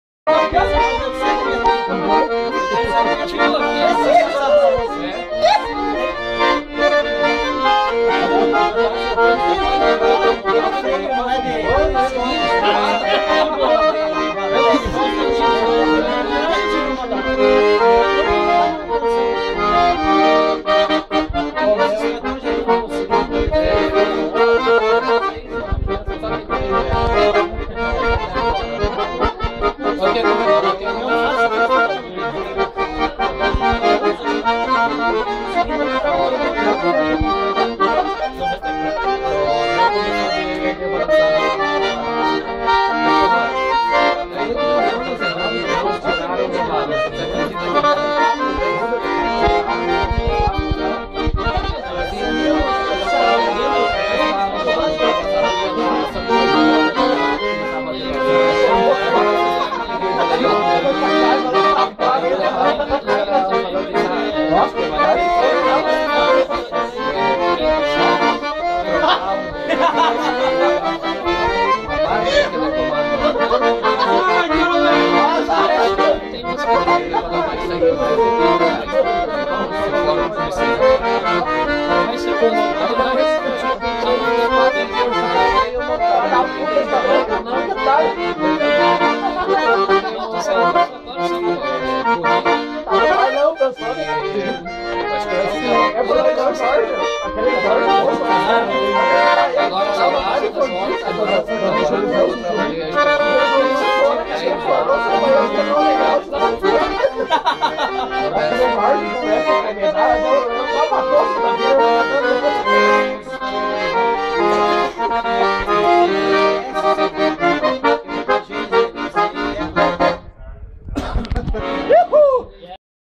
Neri - Esmiussando a gaitita velhaaaa
Gaitaço Aniversário do Zebra - Xixo e batata frita..mazáaaaaaaaaaaaa !! 23/05/2013 - Rio Claro do Sul - PR.. Bar do Nentio